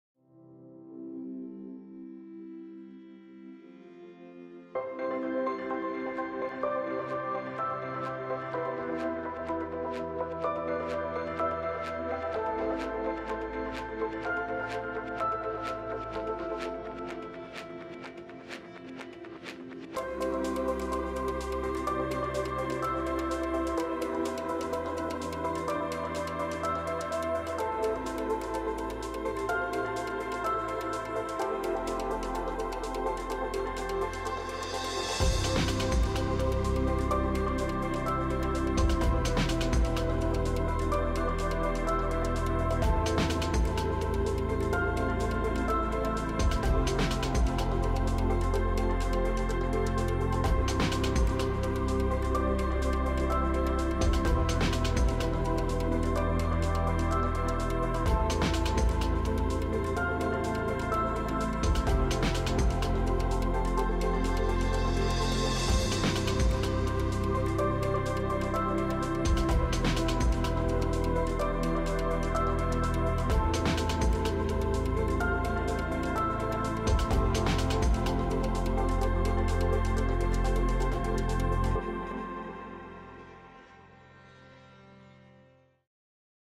Vídeo Comercial ESP - SPHERAG
Vídeo comercial de la plataforma de gestión de agricultura de la compañía Spherag.